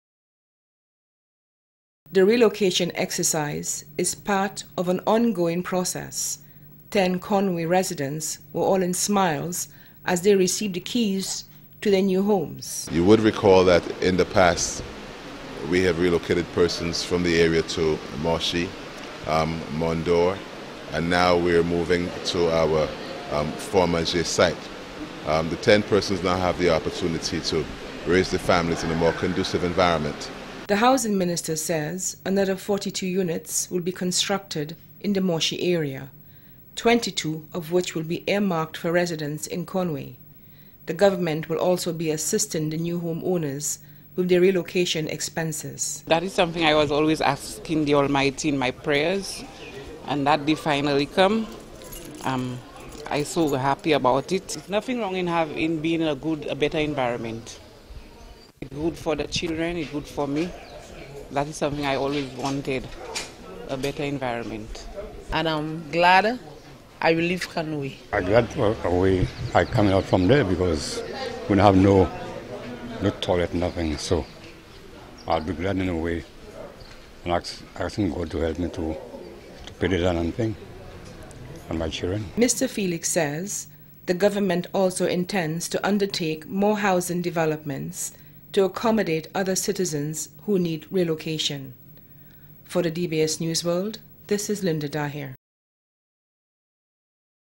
Home Run
Ten Conway residents have received keys to their new homes as part of the government's relocation exercise.